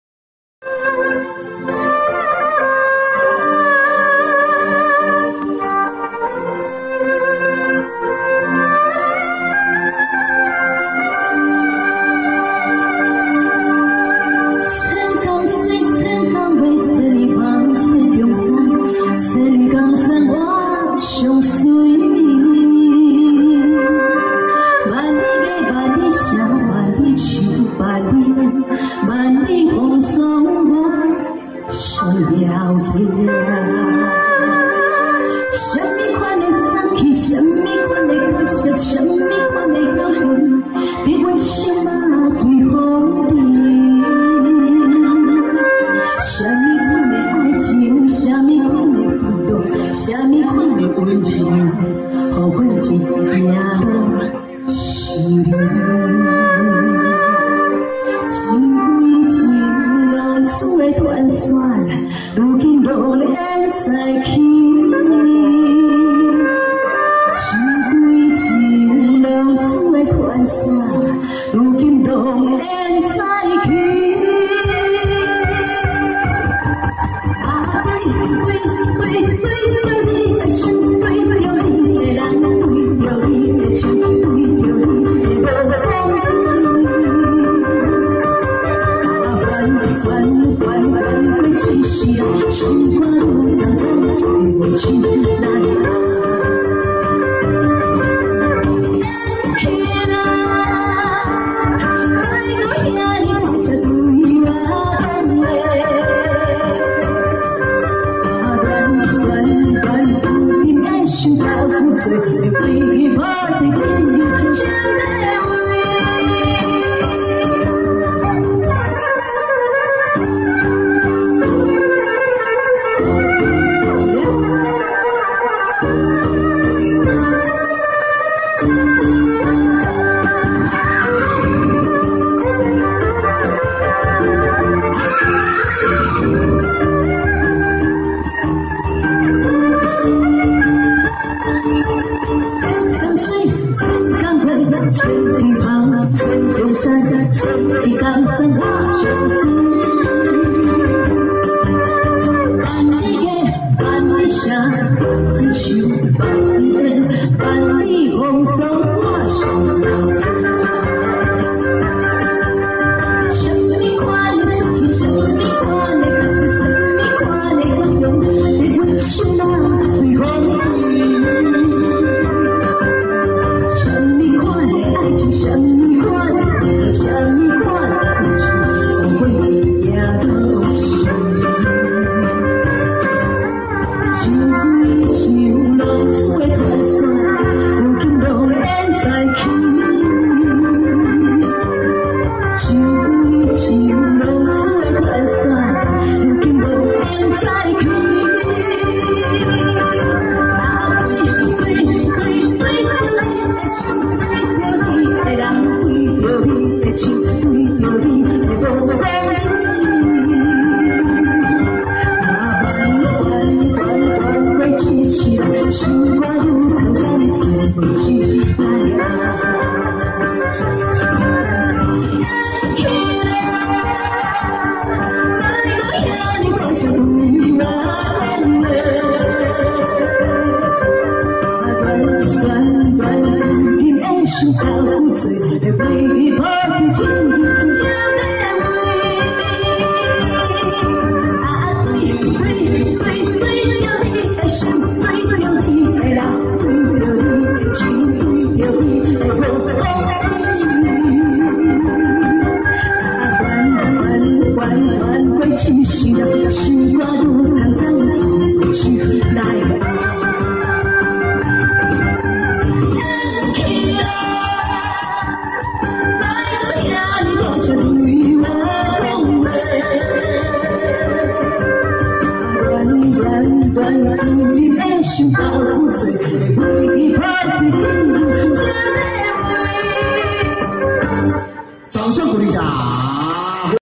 2017 4 23 (日) 新營 都心苑~金華漾 兩大建案 聯合舉辦 LOVE MOM 母親節暖心專案系列活動
執行單位:凱崴創意娛樂事業
              摩力創意娛樂工作室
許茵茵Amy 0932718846